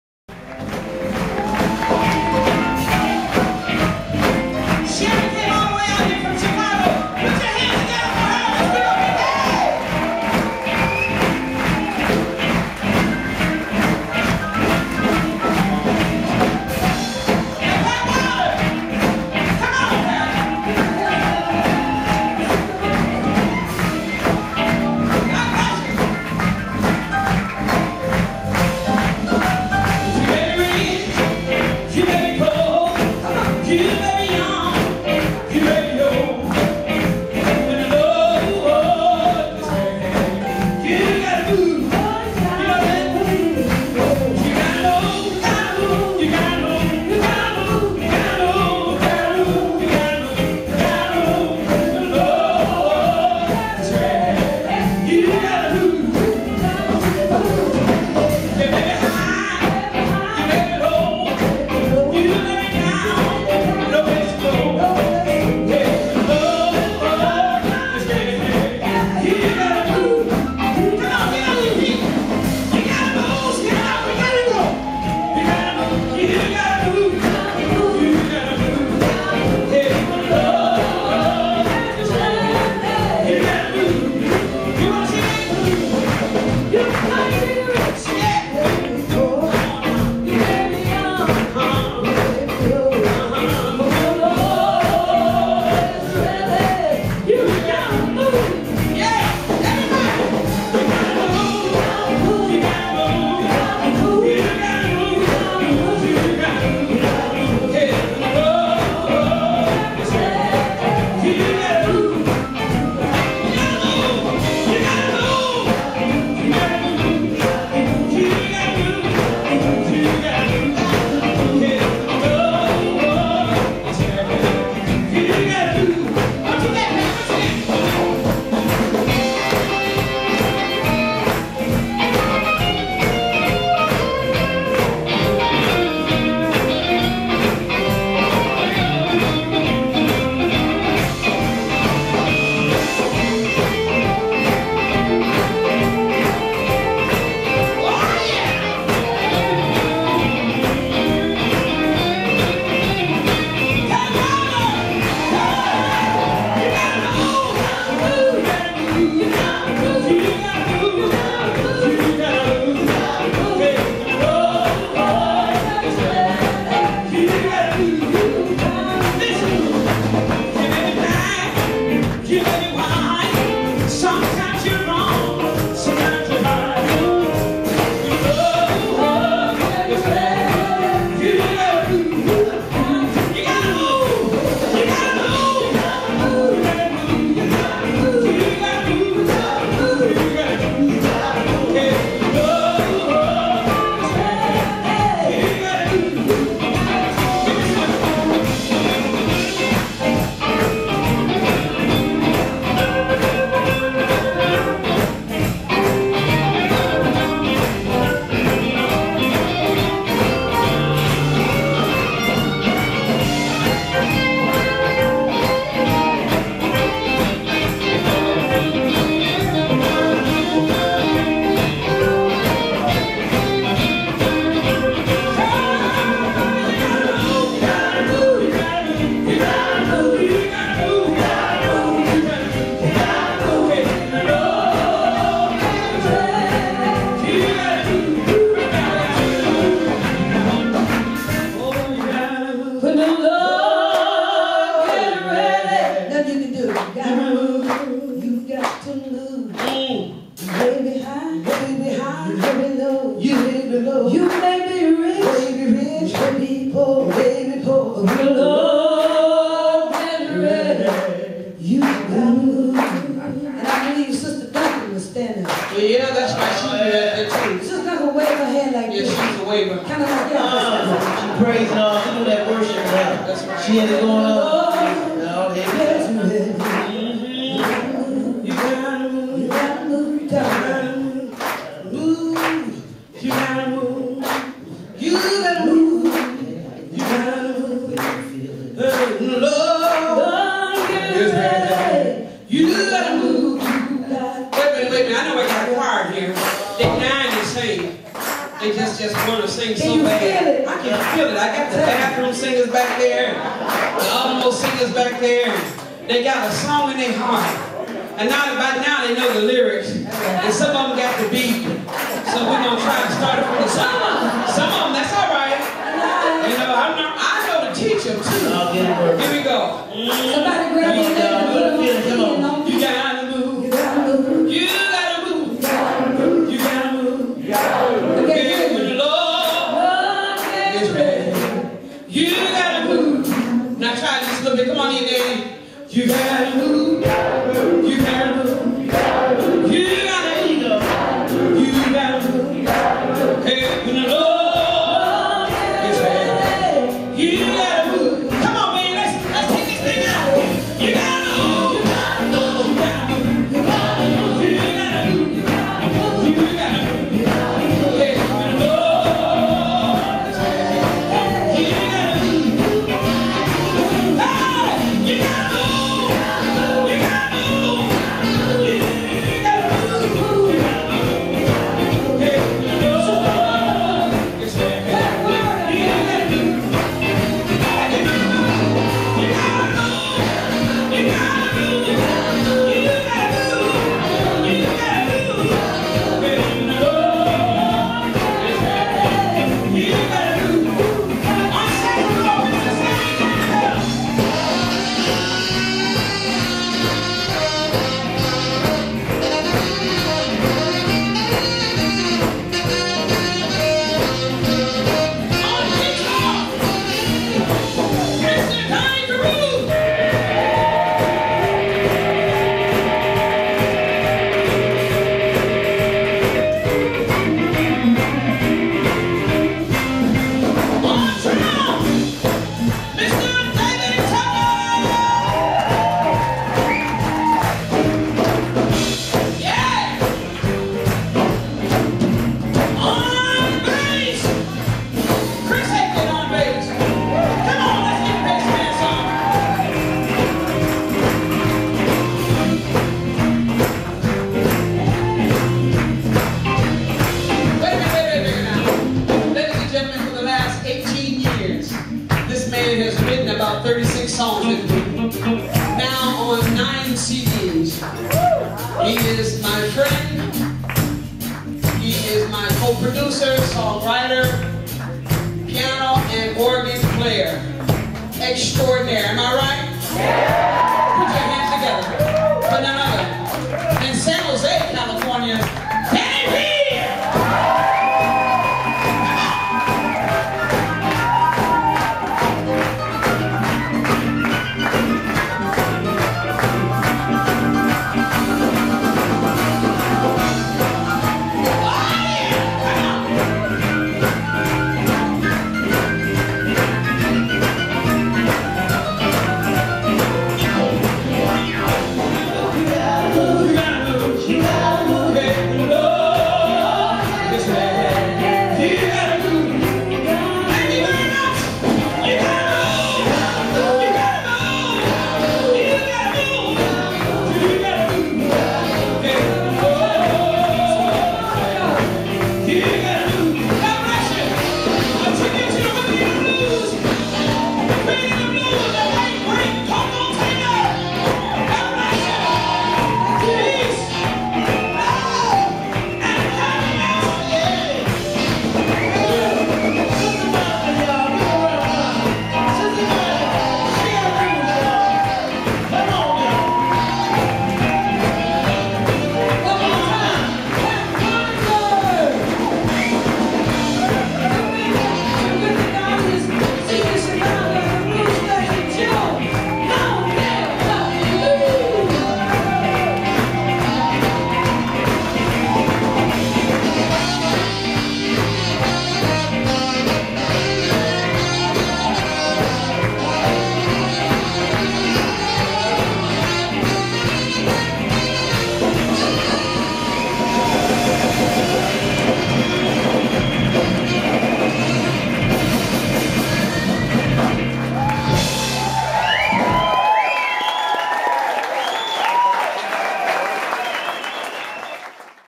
finale - Women of the Blues at Montalvo
Sista Monica, Sharon Lewis and Pat Wilder at the JC Smith production: Women of the Blues at Villa Montalvo 3-19-10